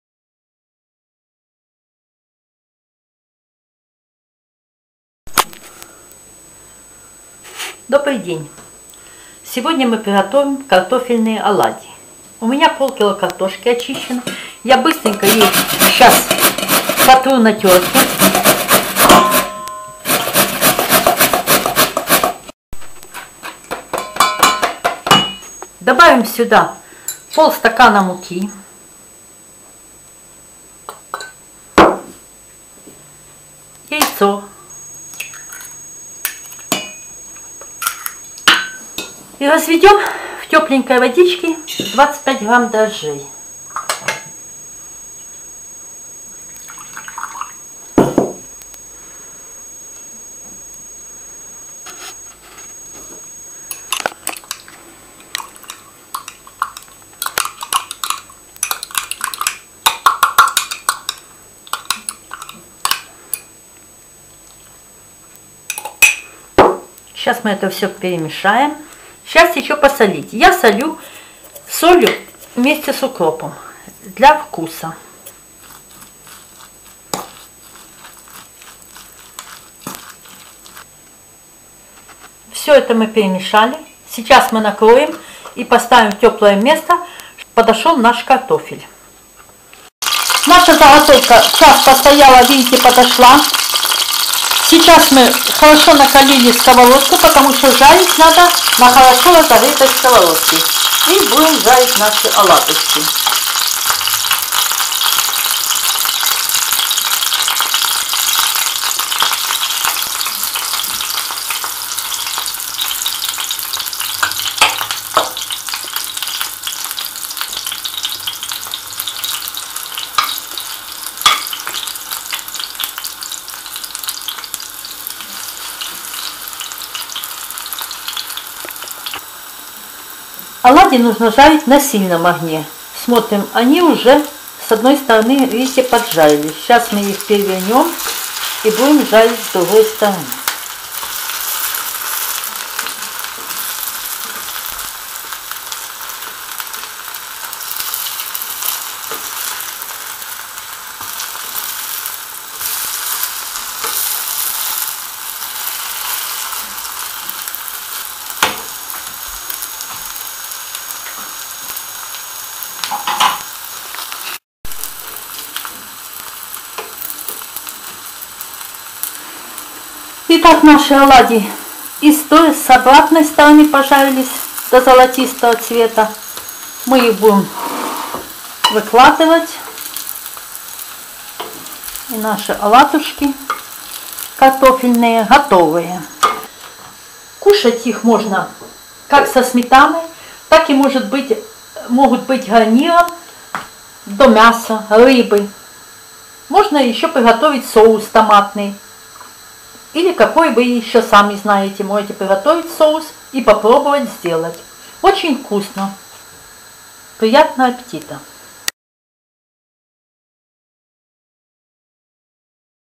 Картофельные оладьи . Potato fritters
Предлагаю рецепт приготовления картофельных оладушек, очень вкусный картофель в таком приготовлении. Приятного аппетита.